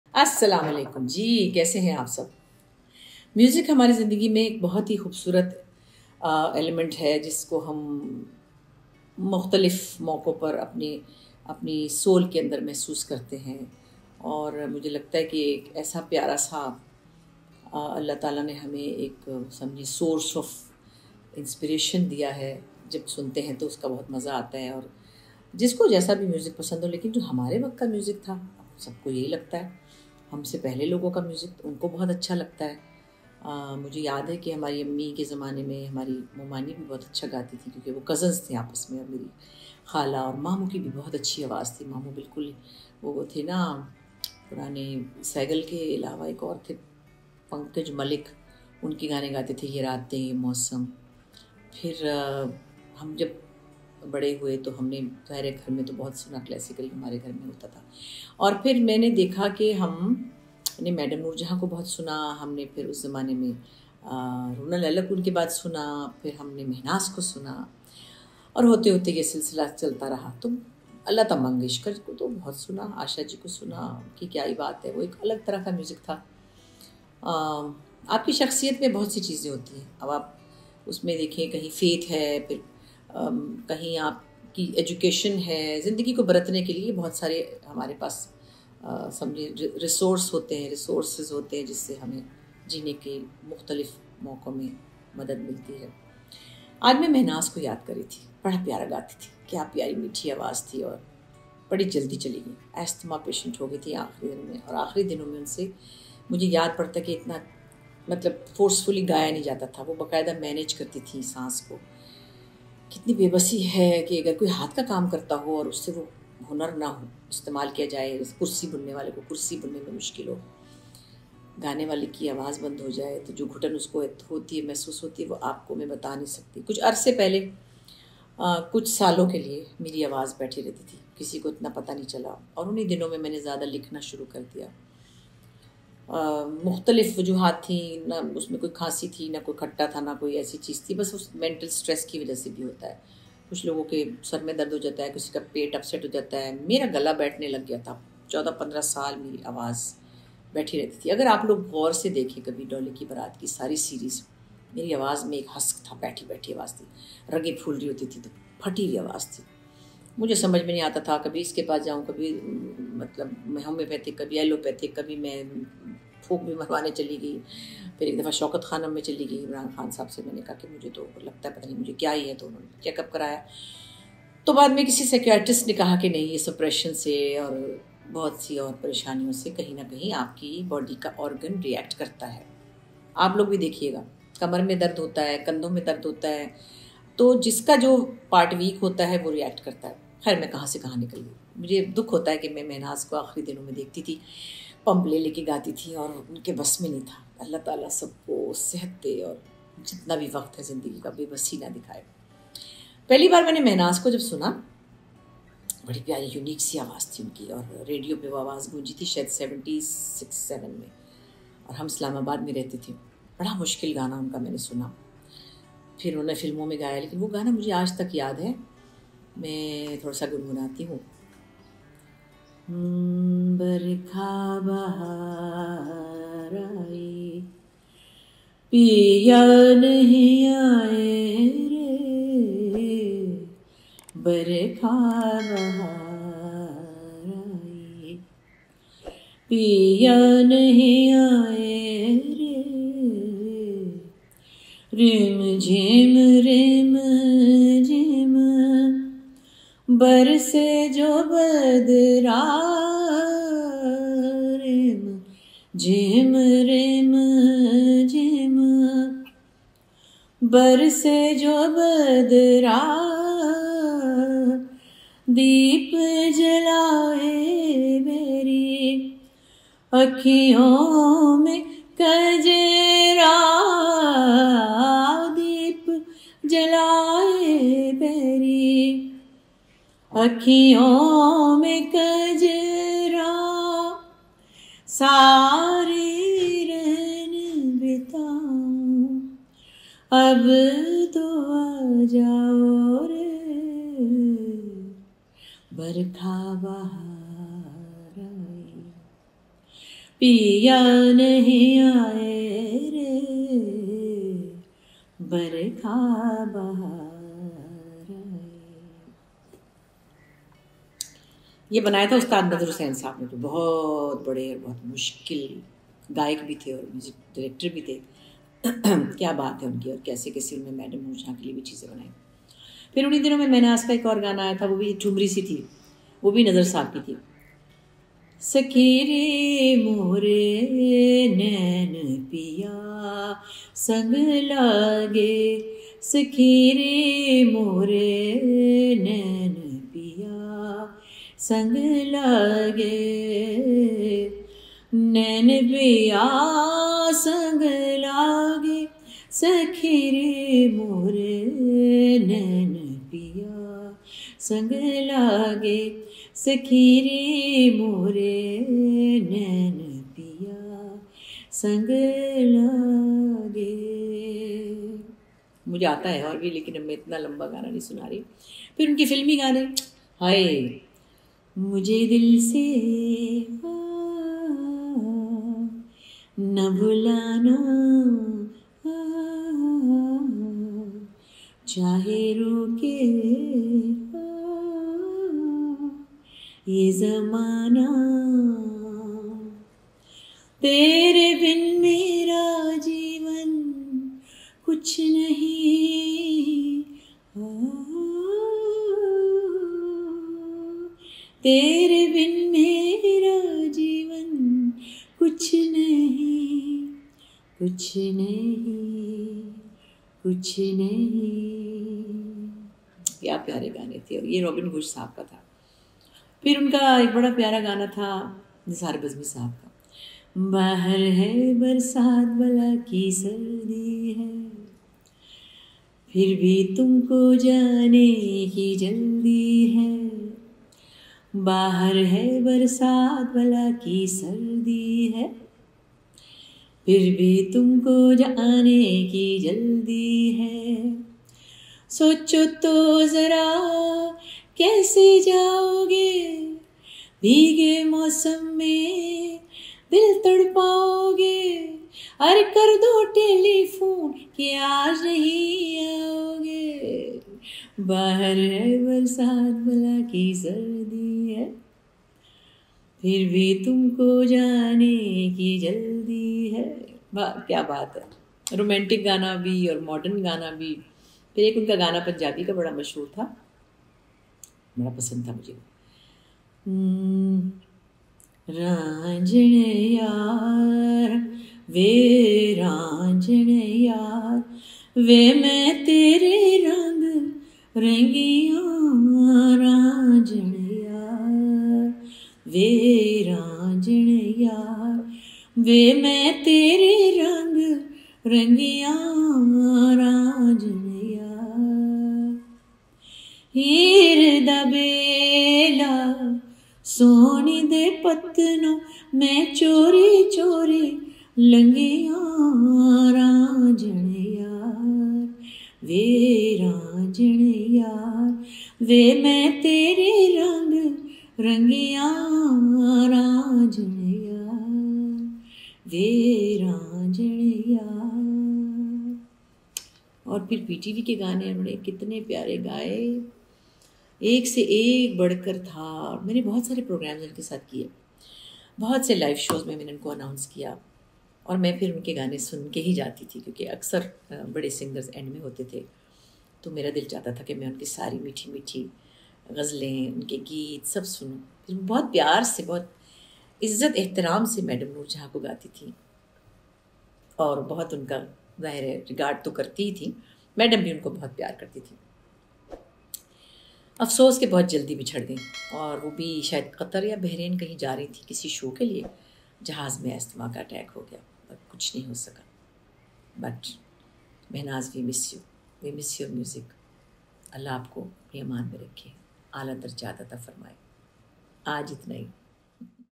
Mehnaz Ki Yaad Aik Meethi Awaz | Bushra Ansari
In this video, I Bushra Ansari is missing the voice of Pakistani Legendary singer Mehnaz Begum and paying tribute to her by singing songs